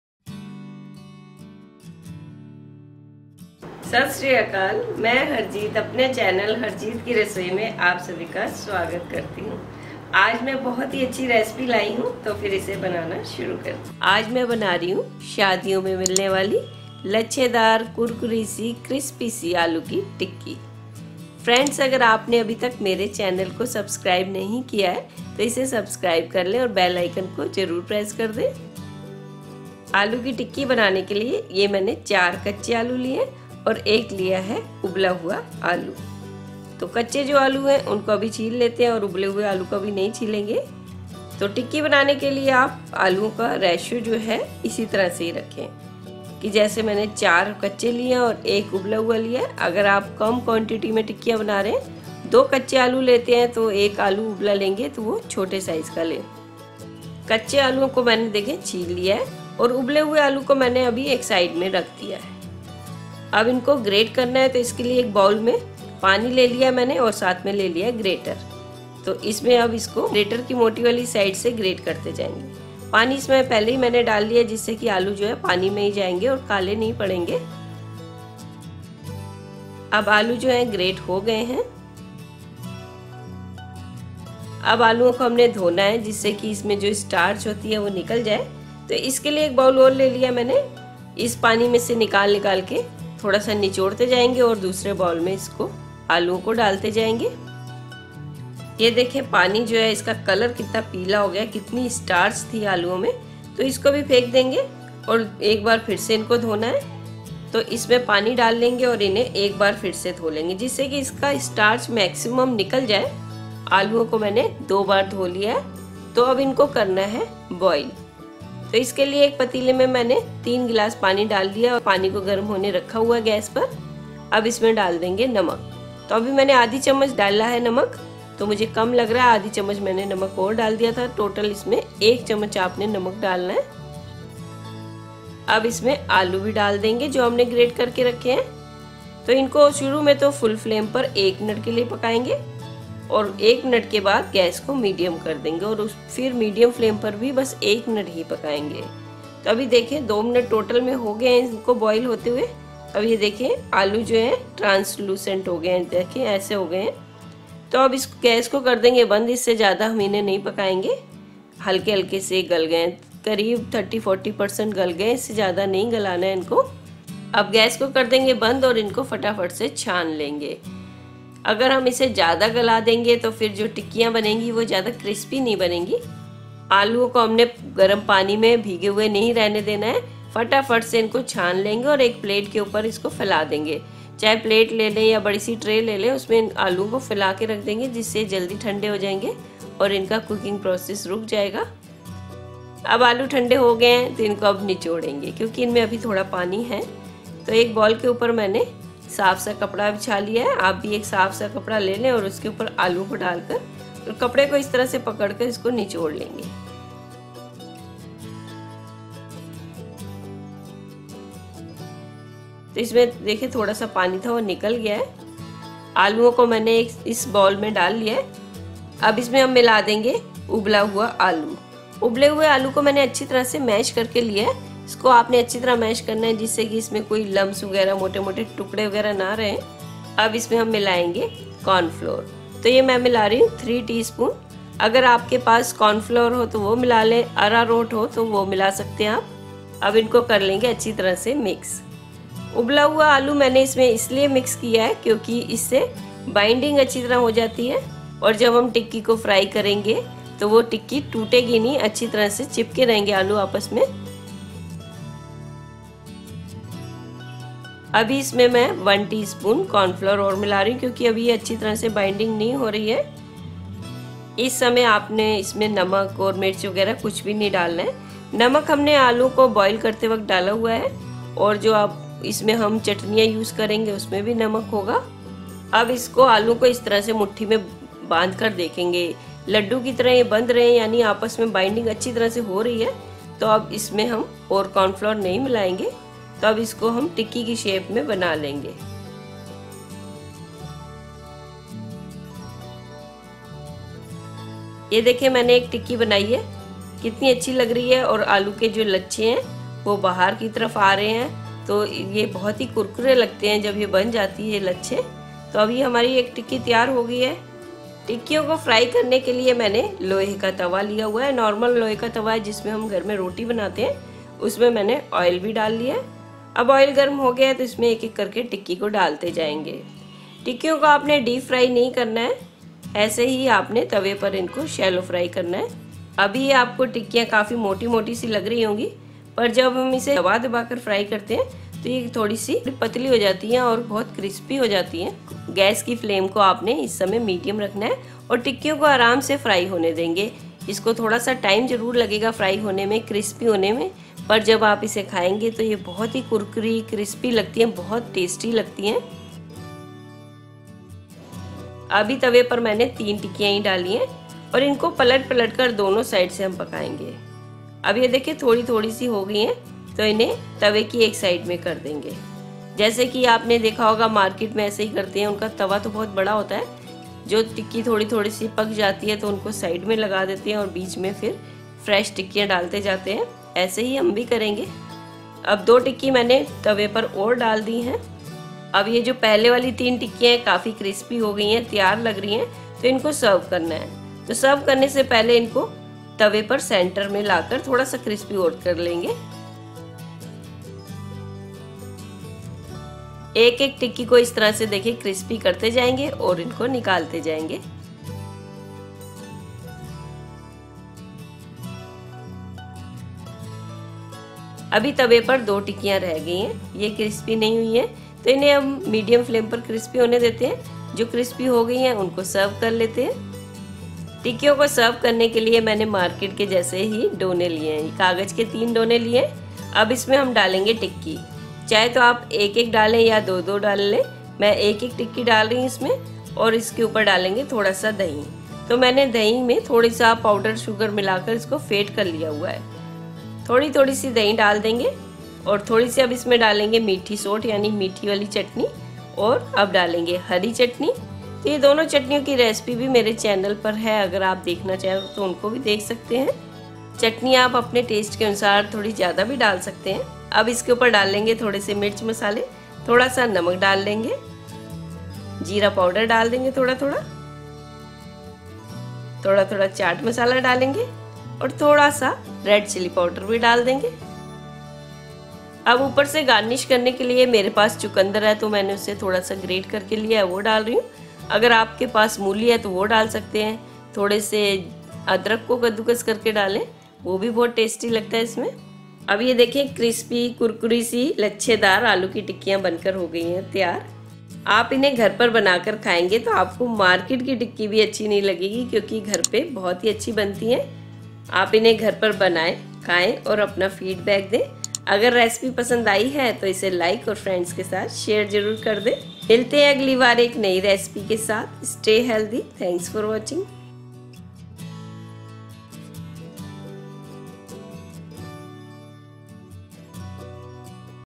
कुरकुरी आलू चाट - लच्छा रोस्टी शादी वाली - aloo roastie chaat hindi-आलू टिक्की चाट-Harjeet Ki rasoi
This is a new aloo snack made by harjeet ki rasoi.This bring to you an easy to make recipe with everyday ingredients.So spice up your life by making this delicious and crispy snack.

Ingredients :
Potato
Oil
Coriander
Red Chilli Powder
Green Chilli
Curd
Sweet Chutney
Green Chutney
Salt
Jeera Powder
Chat Masala
Sugar Beets (Chukundar)